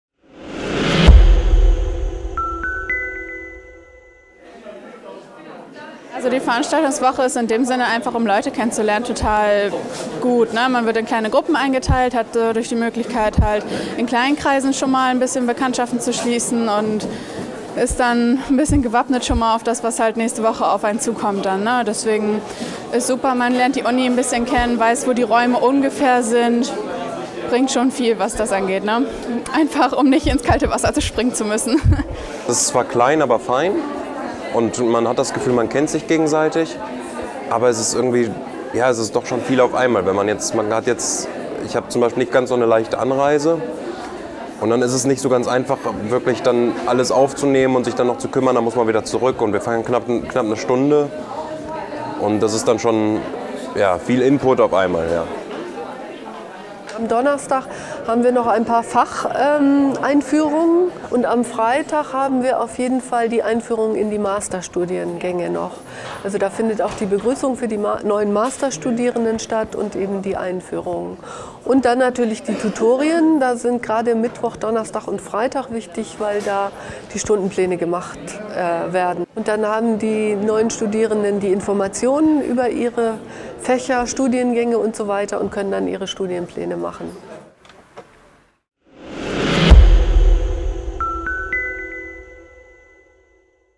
Erstsemesterbeginn an der Universität Vechta
An der Universität Vechta ist die Orientierungswoche in vollem Gange. Für die 1.241 Erstsemester-Studenten heißt es nun, Neuland zu erkunden und Kontakte zu knüpfen. Wie das bislang gelingt, erfahrt ihr in diesem Video.